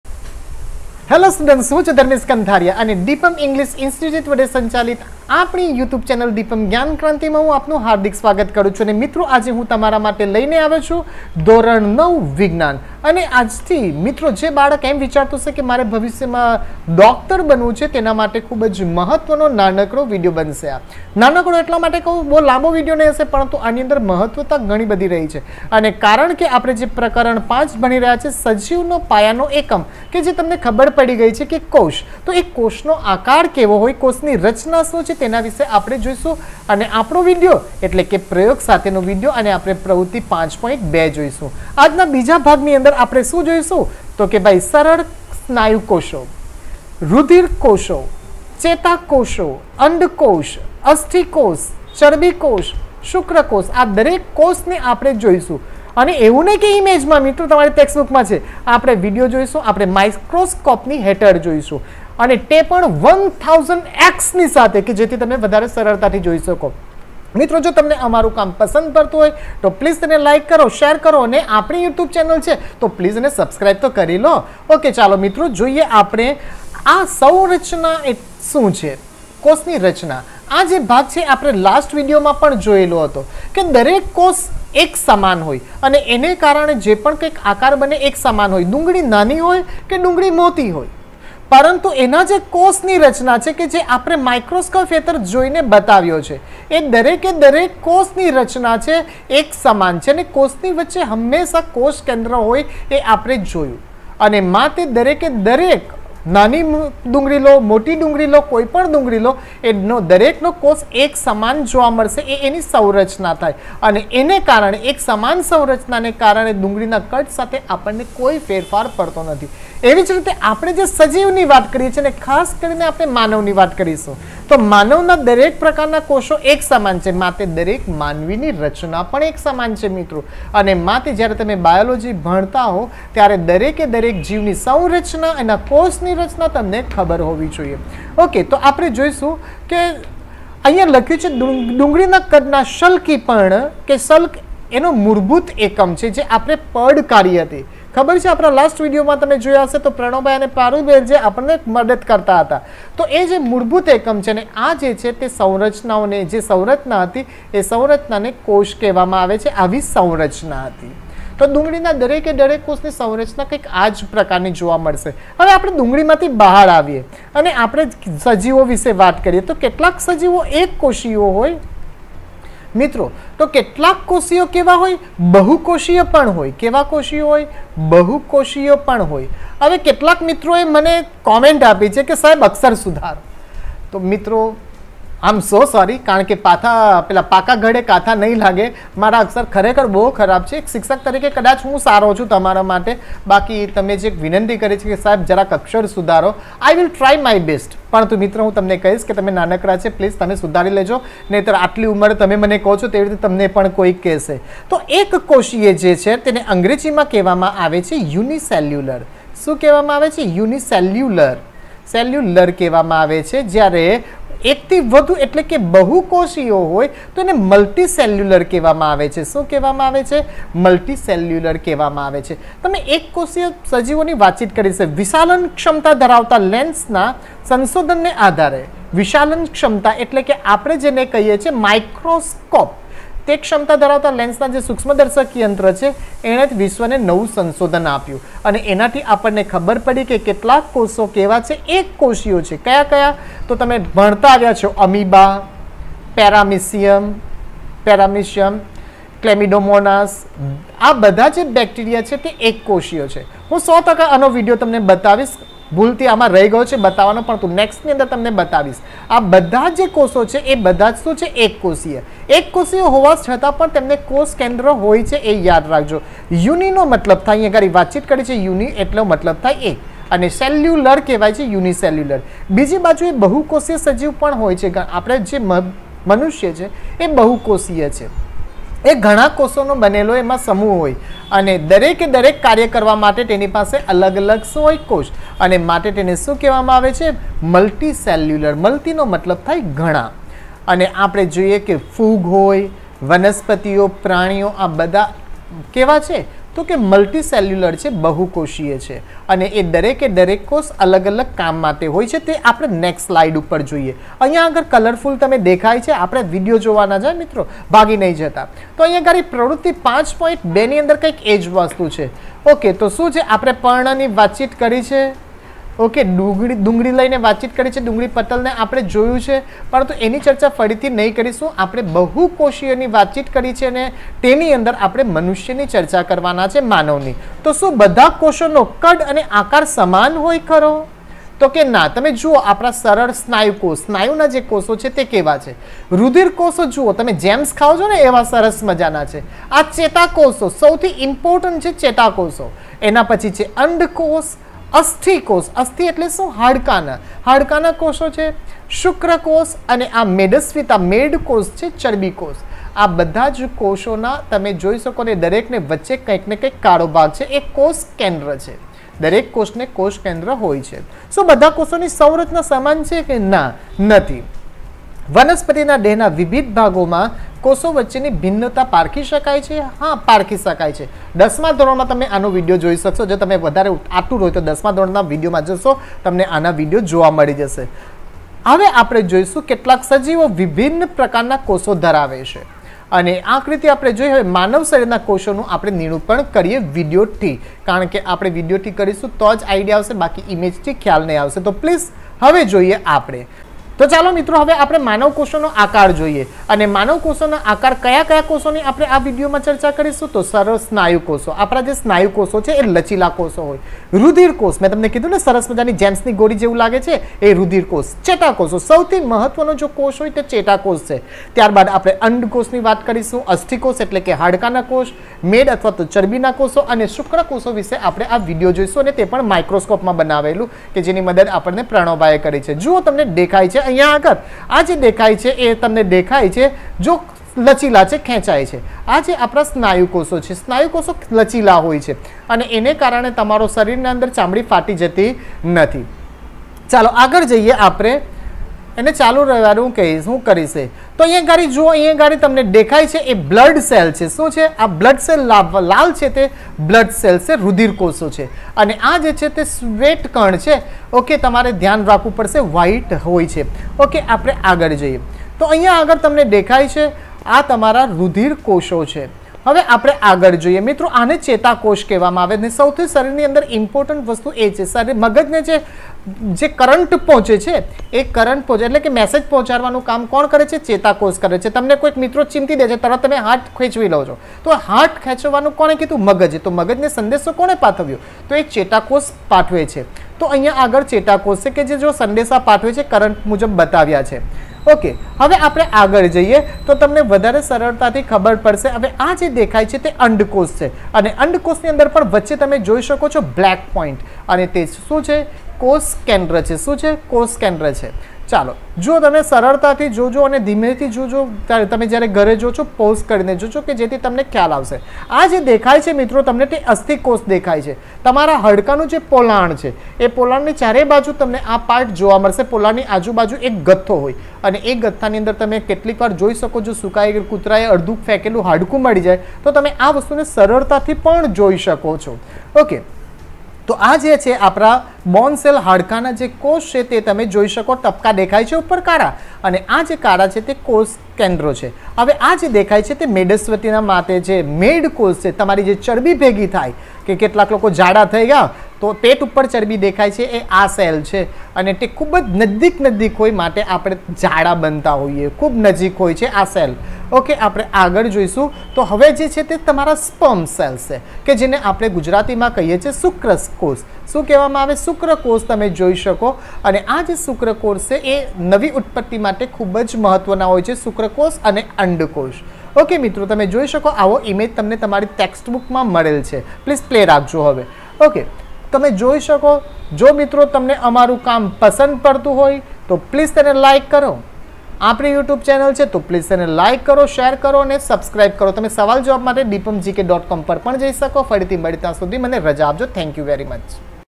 STD 9 || SCIENCE || CH 05 || સજીવનો પાયાનો એકમ || Video-02
આ વિડિયો દ્વારા તમે  CH 05 સજીવનો પાયાનો એકમની માહિતી મેળવી શકશો... ધોરણ 9 ની NCERT Textbook પદ્ધતિ  પ્રમાણે ખુબજ સરળતાથી તમે બધા ટોપિક સમજી શકશો...